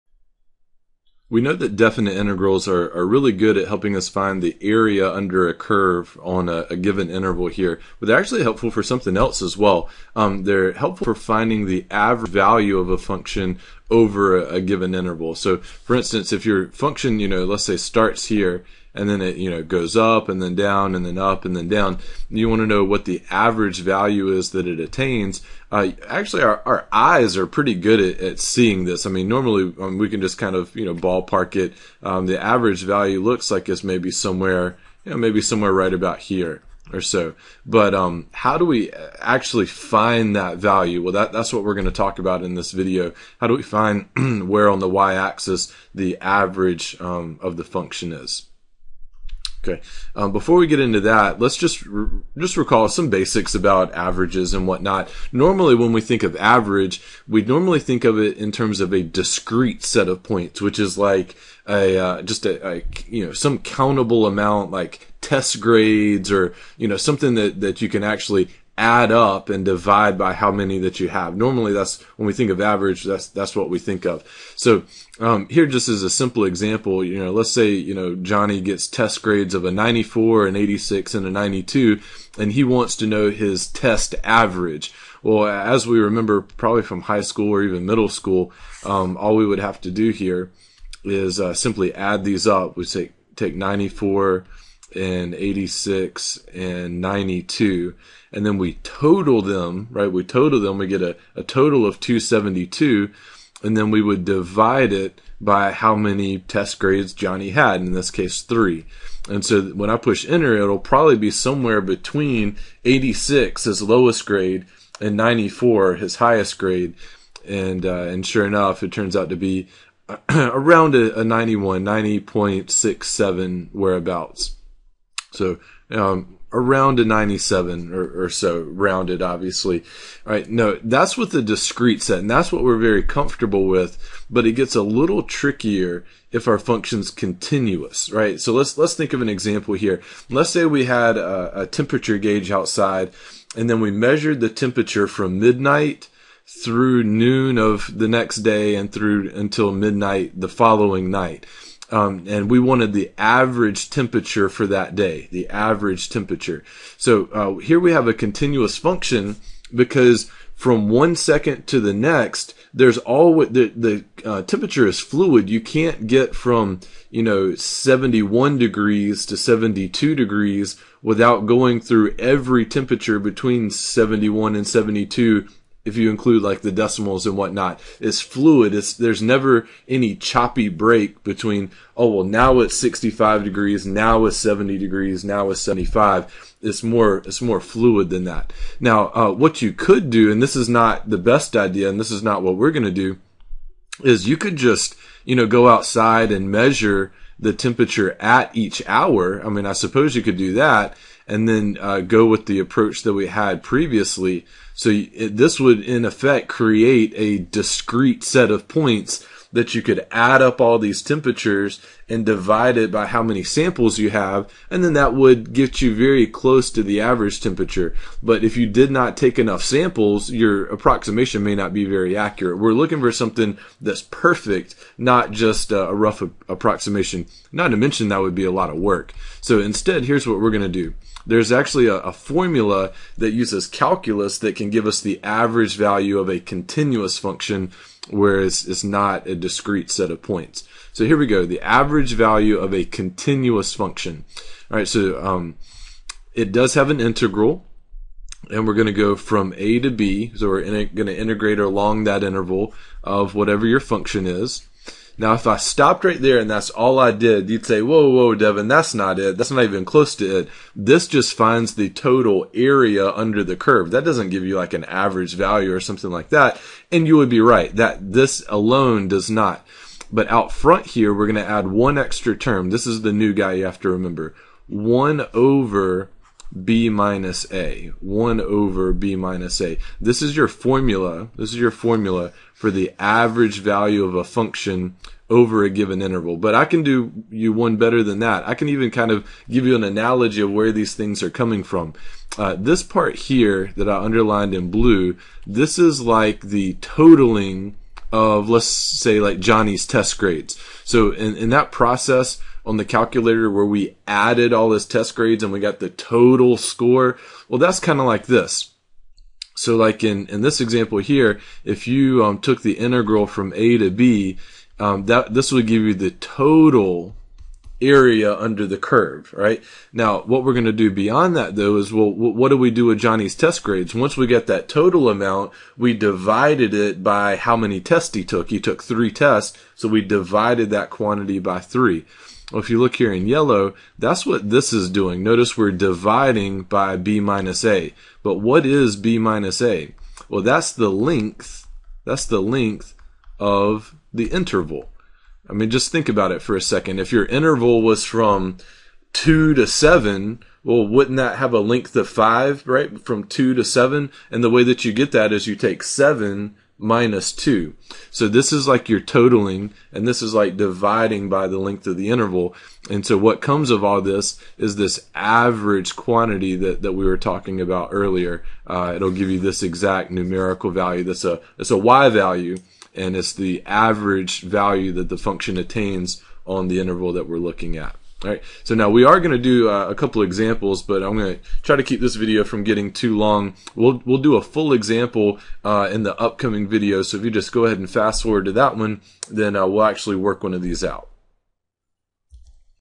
Average Value of a Function Over an Interval
If you enjoyed this video, take 30 seconds to find hundreds of free, helpful videos. No registration required! How to compute the average value a function attains over a closed interval.